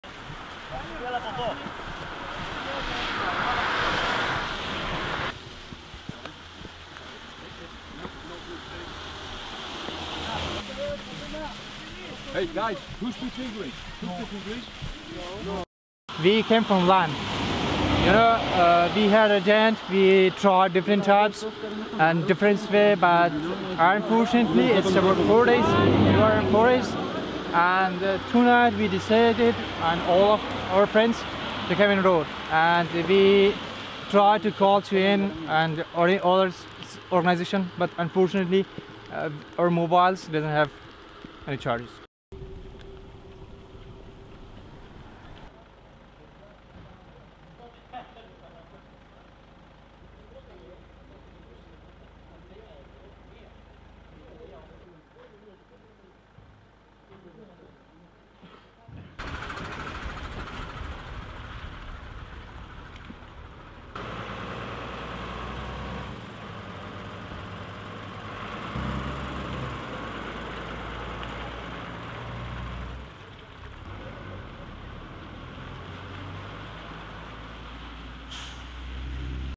Afghan and Somali migrant trek across Greek border
(28 Feb 2020) Greece reinforced security measures at its sea border with Turkey on Friday, as it tried to stop a new wave of migrants from entering the European Union after Turkey said it would no longer hold them back.
Security forces temporarily sealed off the Kastanies border crossing in the northeastern Evros area.
Greek police officials said that around 1,200 people gathered on the Turkish side of Kastanies, northwest on Evros region, late Friday and have tried to push through the locked gates several times.
Some managed to cut holes in the border fence close to the crossing and attacked police with stones but were repulsed with tear gas and stun grenades.
A police officer told The Associated Press that pressure was mounting along the 200-kilometre (125-mile) land border from migrants trying to force their way through, and groups were being constantly repulsed.